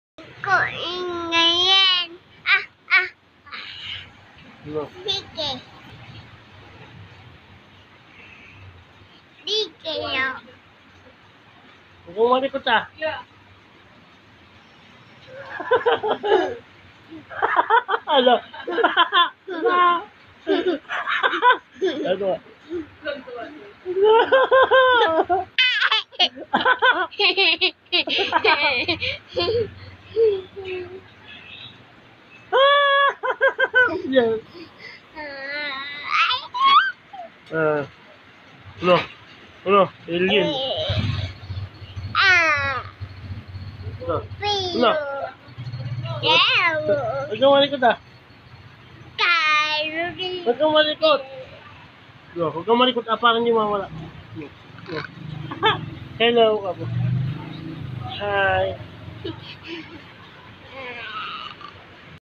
Laugh trip si kid sobra kulit..pro mas makulit ung nag video sa last. (WATCH) till the end
Dami tawa asian boy..